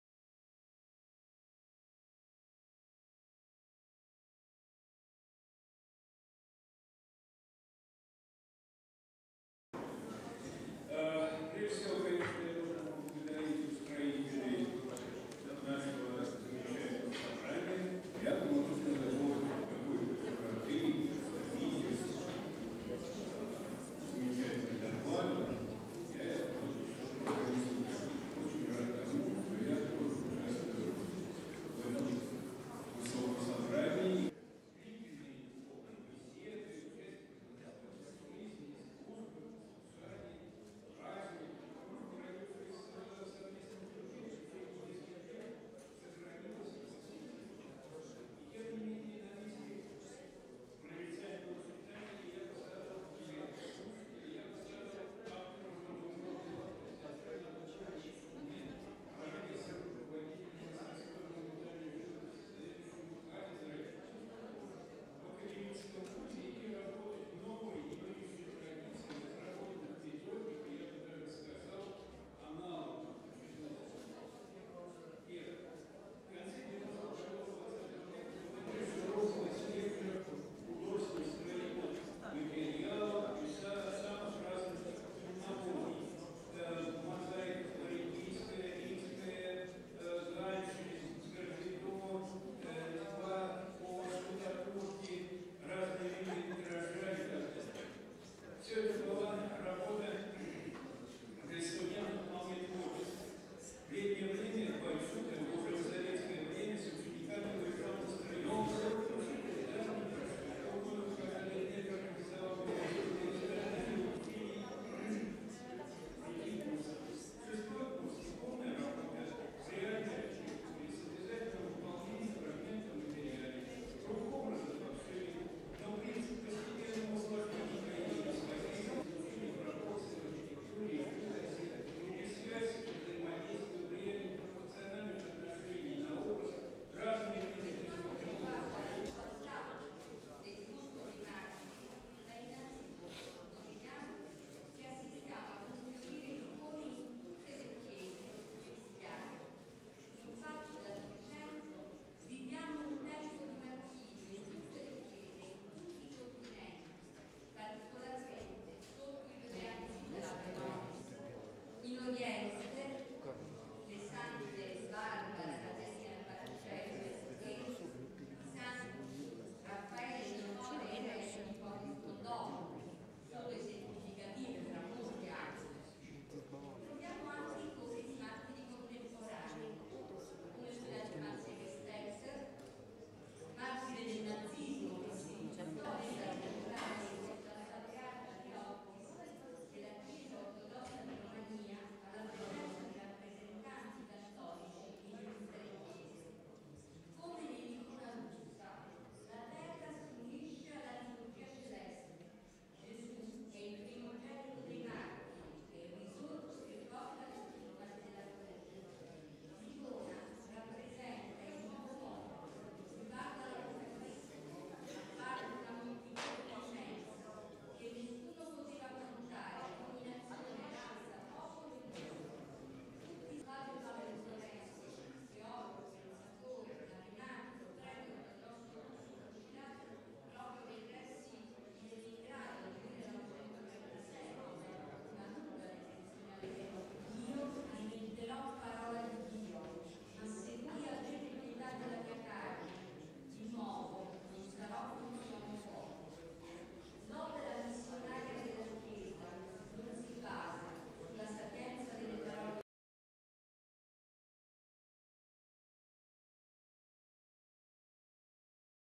Damianakis_brief_workshop.mp4
Elias Damianakis icon-painting workshop at the Modern Icon Conference, November 9-11, 2011 (St. Petersburg, Russia)